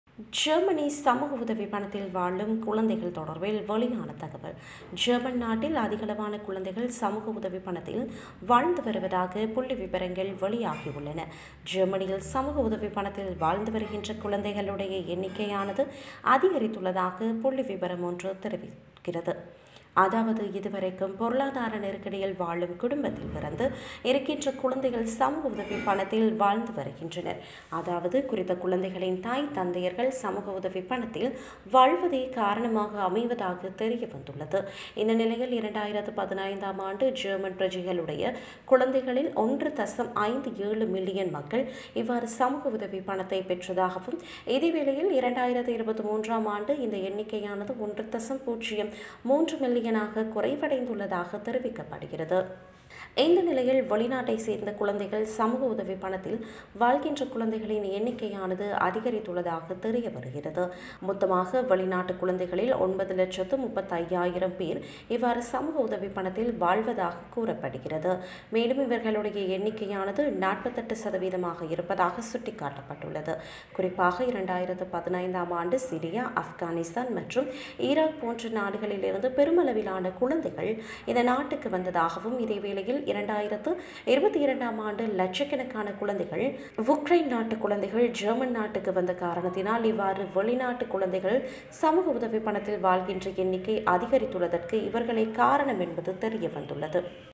ஜெர்மனி சமூக உதவி பணத்தில் வாழும் குழந்தைகள் தொடர்பில் வெளியான தகவல்
ஜெர்மனி நாட்டில் அதிகளவான குழந்தைகள் சமூக உதவி பணத்தில் வாழ்ந்து வருவதாக புள்ளி விபரங்கள் வெளியாகியுள்ளது.